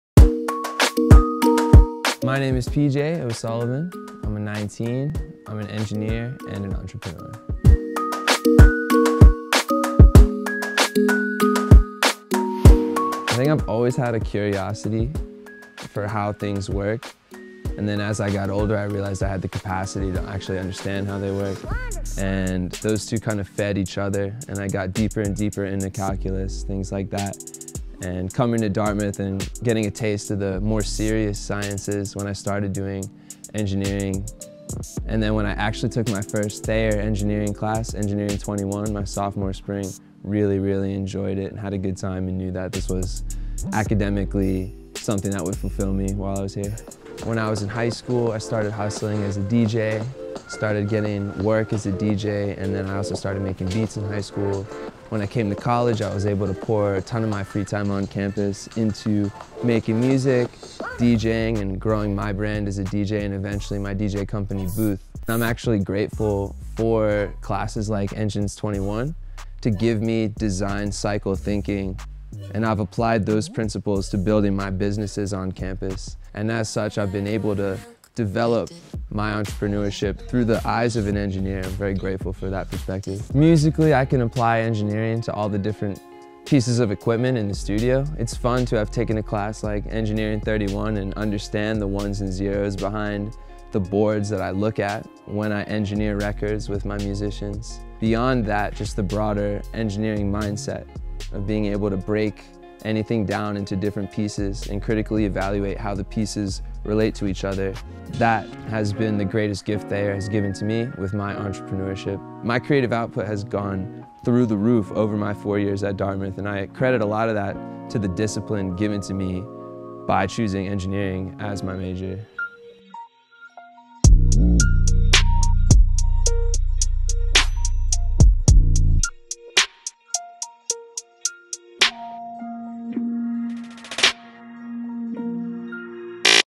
I'm an Engineer and an Entertainment Entrepreneur
PJ O'Sullivan '19 — founder of the DJ company, Booth — discusses balancing his appreciation for music, deejaying, science, and engineering.

Video edited by Rusty Spydell. Footage provided by PJ O'Sullivan, ig: @juulgrams